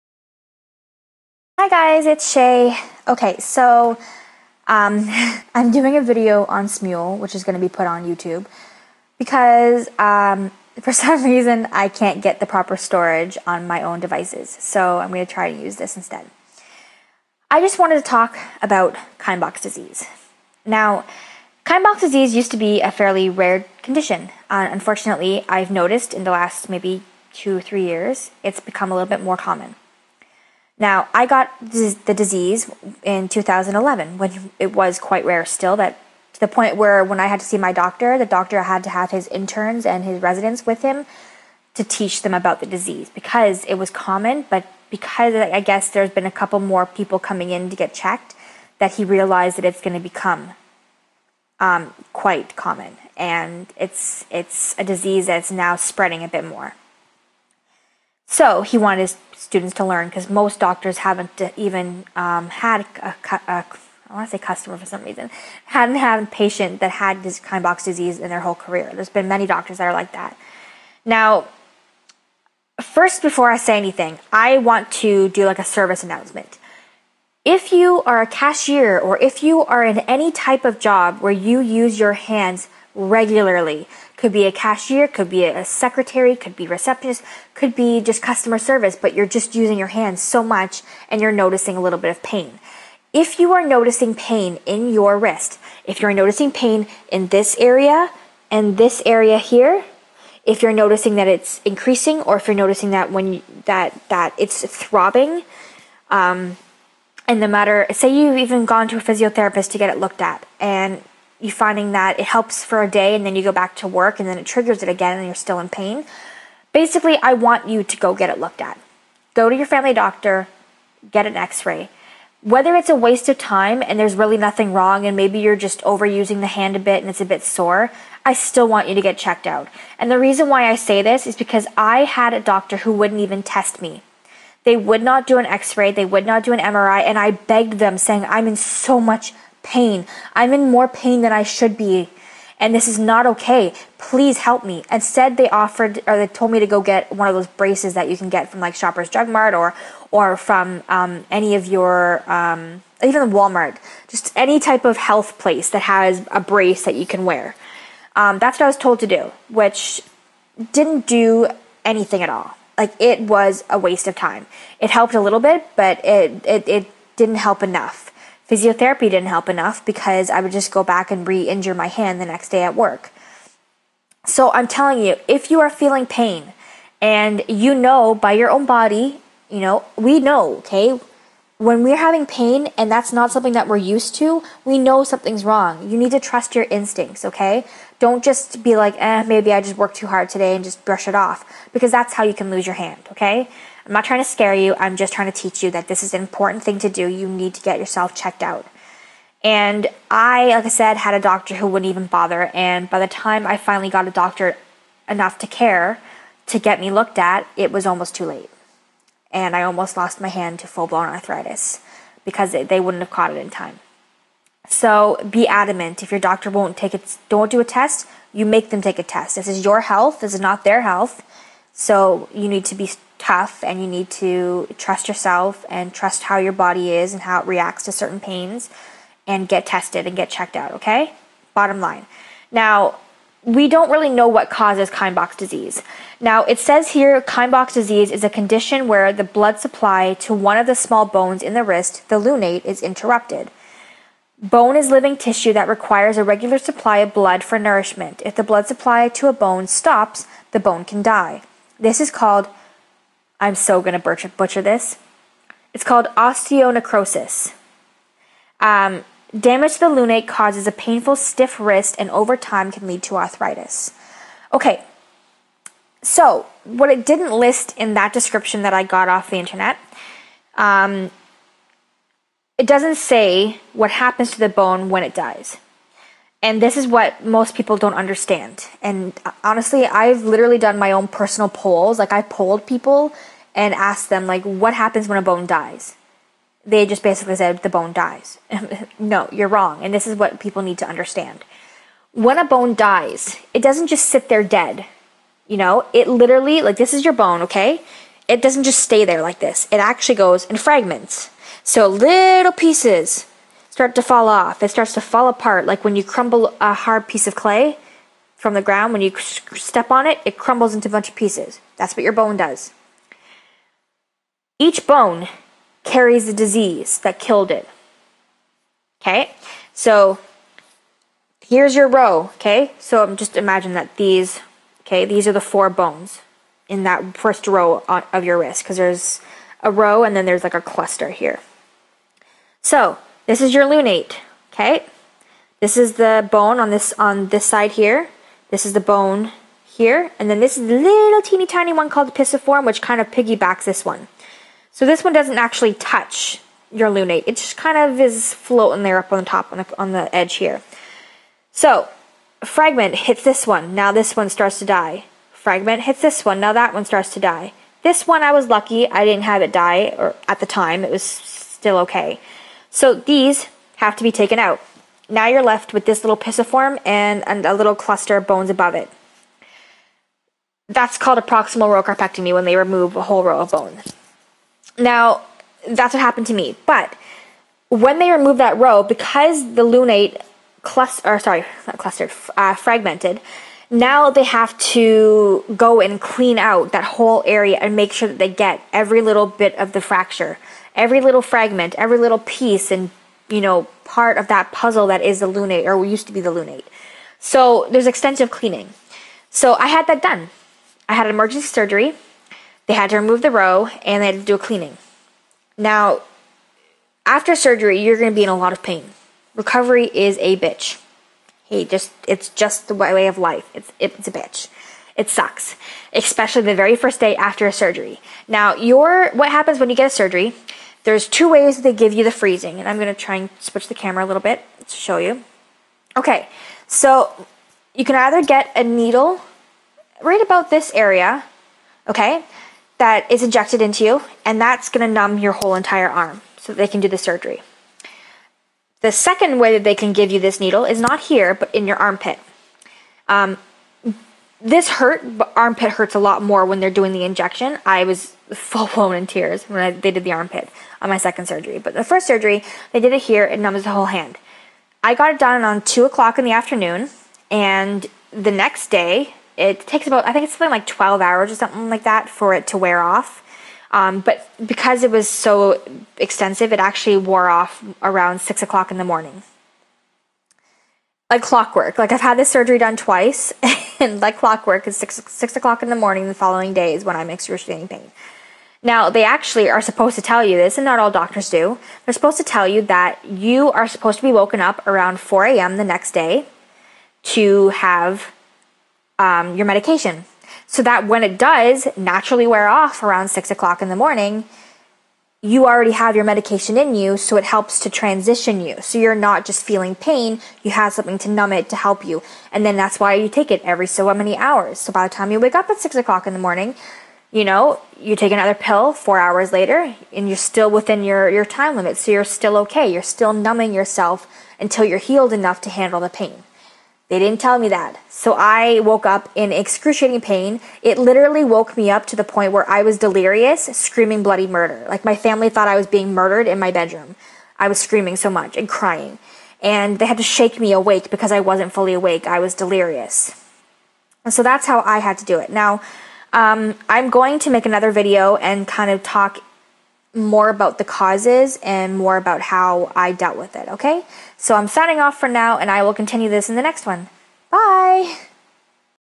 Lets talk about Kienbocks Disease part 1
Hey all! Shay here, i just wanted to share with you my Kienbocks Disease journey and how i dealt with it and how i learned to cope and some tricks and tips i wanted to share with you to help you on your journey with this disease